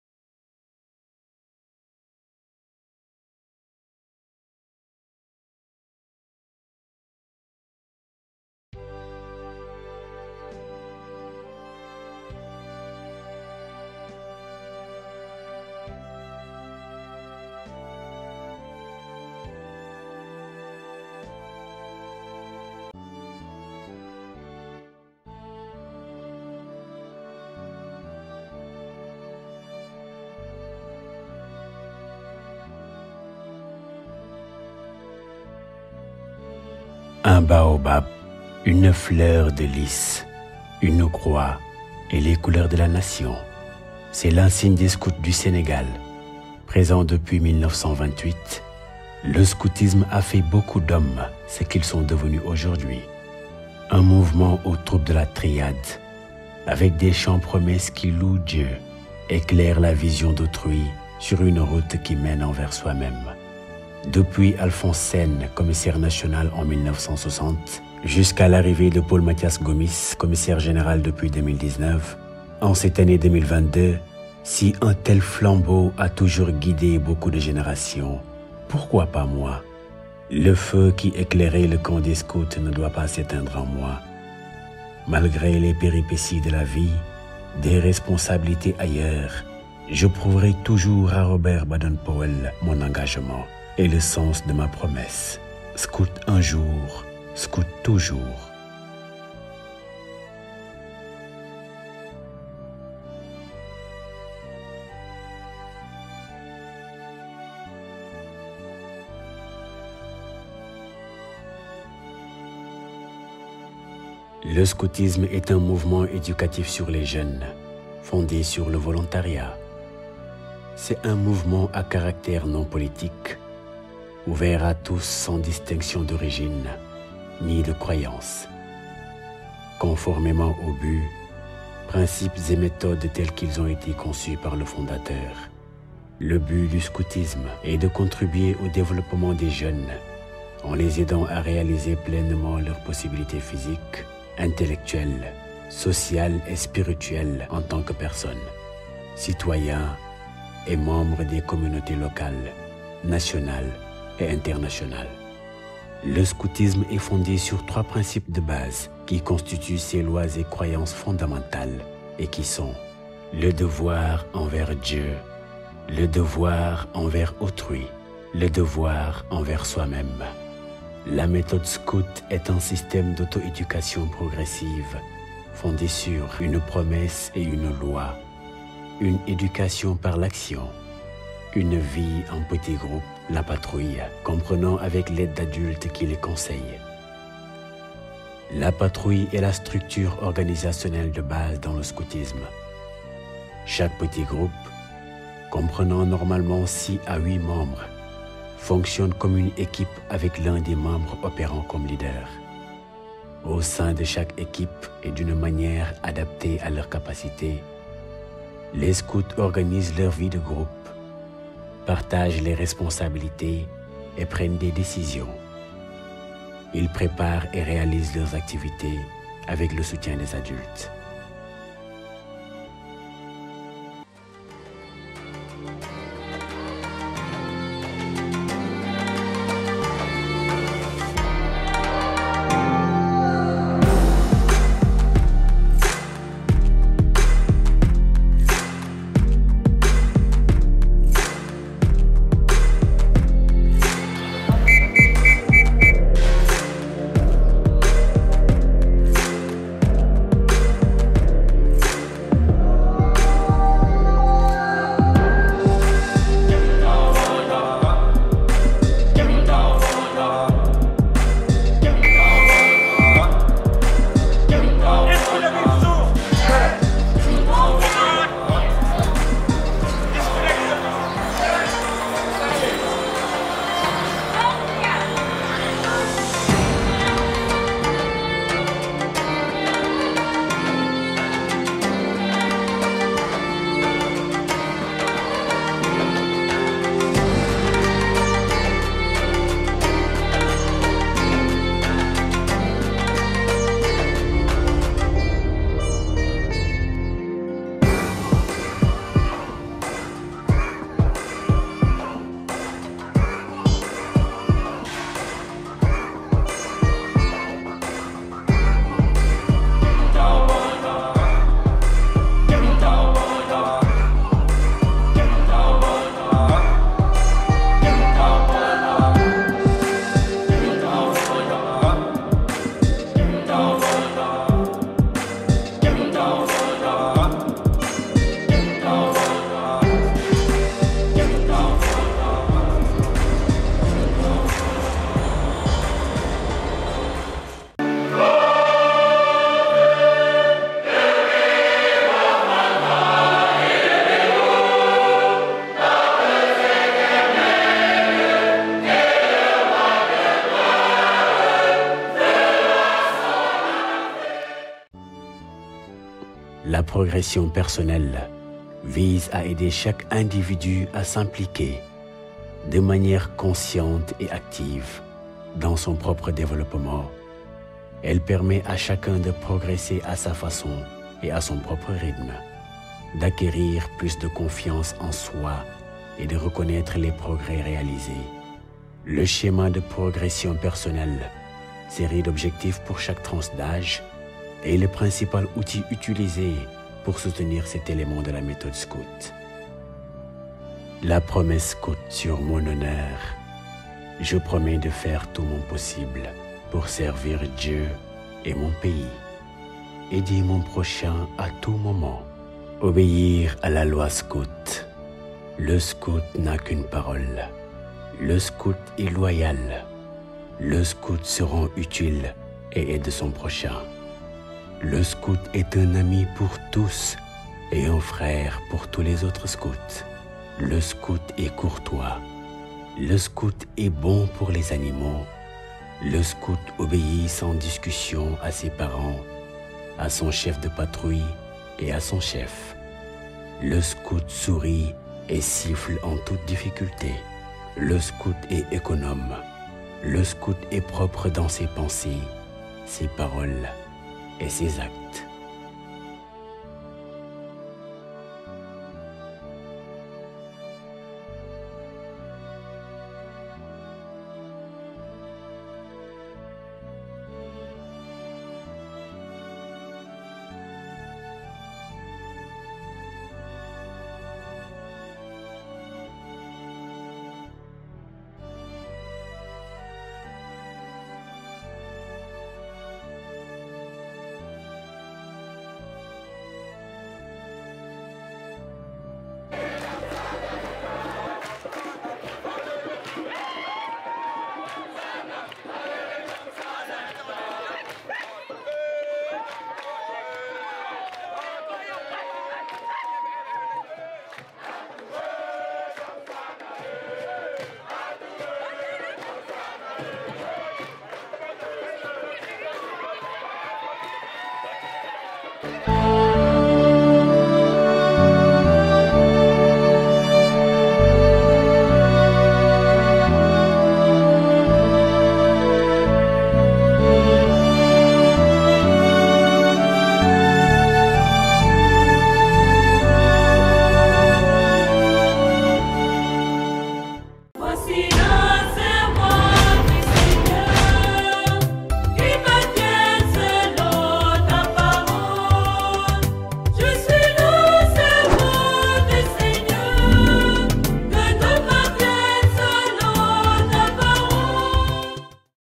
⚜️Scout un jour⚜️
"Scout un jour", une réalisation qui a pour but de rendre un vibrant hommage à "un mouvement de pure formation" mais aussi pour rappeler les valeurs du scoutisme, méprisées par beaucoup de jeune d'aujourd'hui.

"Si chaque jeune sénégalais avait la chance d'intégrer un mouvement d'action catholique tell que le scoutisme,  nous en serions pas là aujourd'hui à parler de manque de civisme et de citoyenneté de la part des jeunes" disait Théodore Adrien Sadio NDIAYE, le représentant du ministre de la jeunesse lors de l'institutionnalisation de la journée du 08 Mai (2021) en honneur des Ainés Scouts.

Mettre en pratique les valeurs du scoutisme, c'est avoir une bonne conduite, une vie sociable, se sentir en phase avec soi-même et participer au développement de son pays... car le SCOUTISME est une école de vie et d'éducation à la citoyenneté: scouts un jour, scouts toujours.